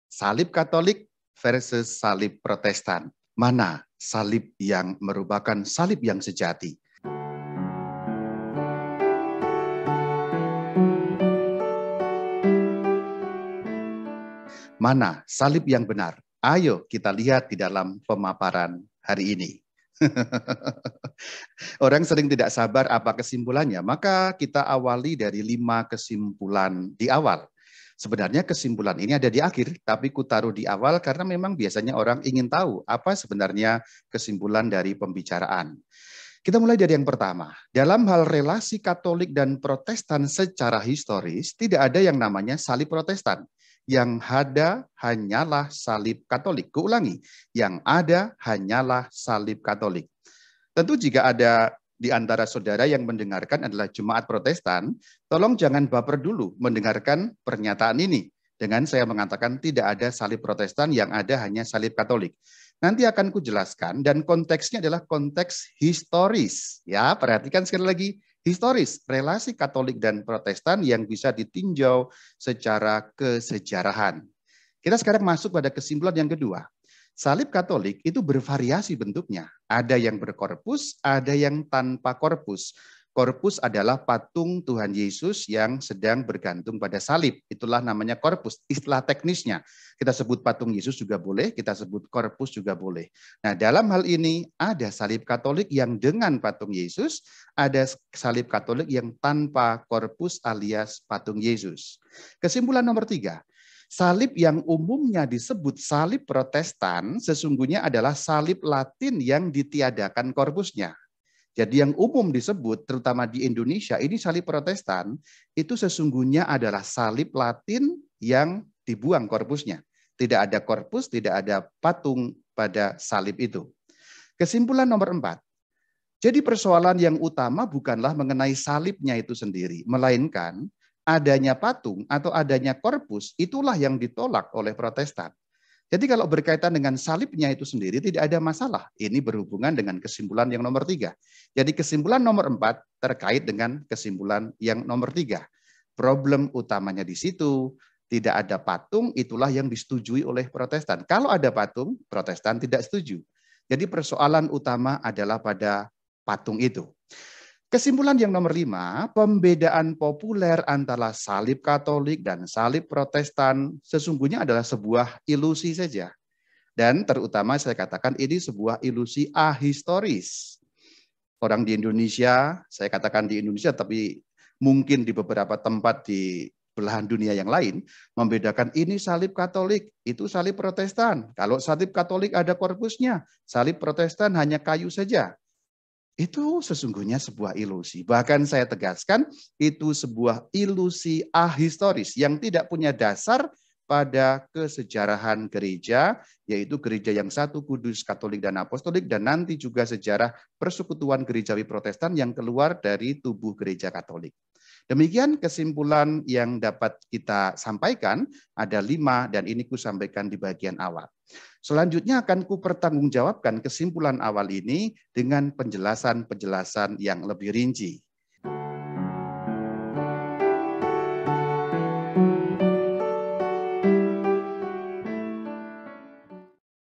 Kesimpulan Penelitian Salib: Katolik vs Protestan
Benarkah ada salib Katolik dan ada salib protestan? Jika ada keduanya, manakah salib yang benar? Tontonlah kesimpulannya di sini.